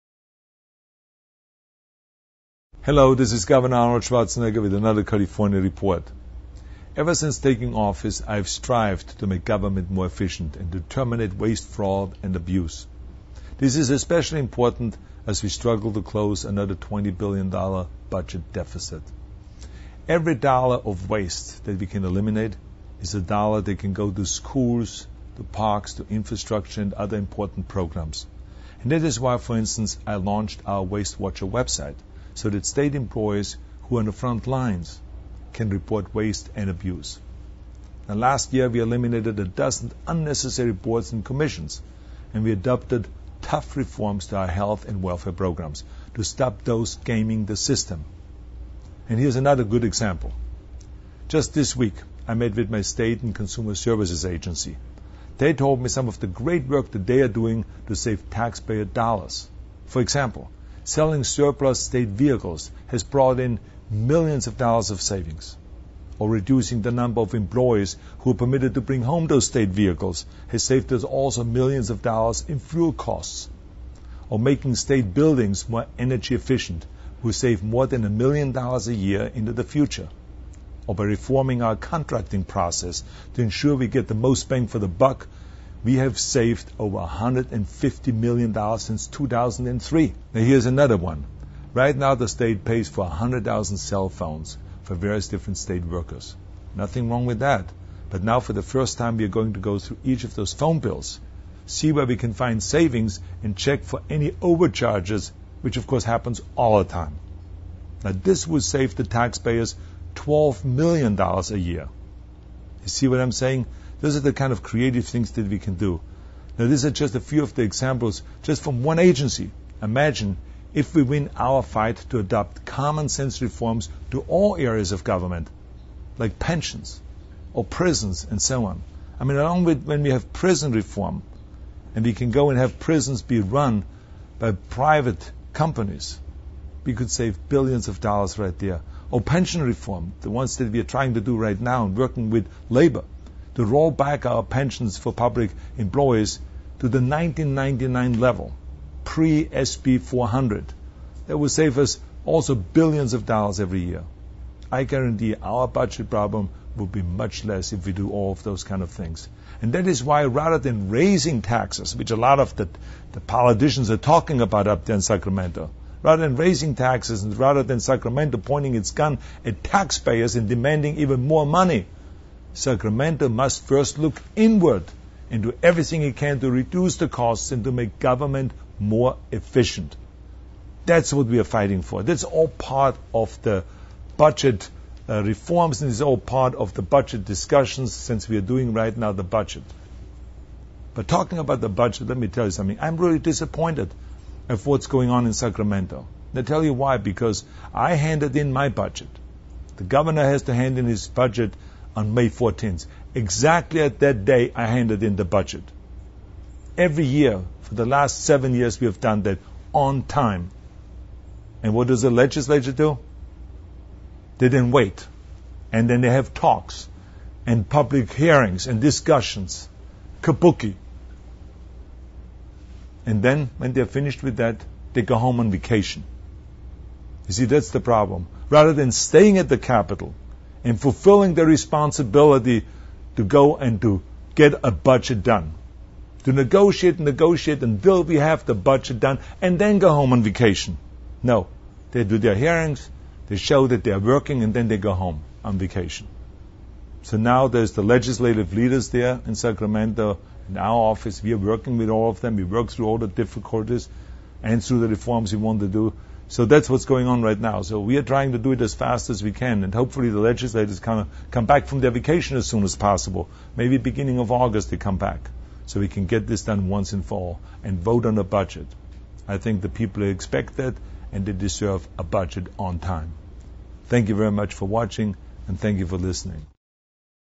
Common Sense Government - Weekly Address - 07/23/2010
Ever since taking office I have strived to make government more efficient and terminate waste, fraud and abuse. This is especially important as we struggle to close another $20 billion budget deficit. Every dollar of waste that we can eliminate is a dollar that can go to schools to parks to infrastructure and other important programs. That is why, for instance, I launched our Waste Watchers website. So state employees who are on the front lines can report waste and abuse.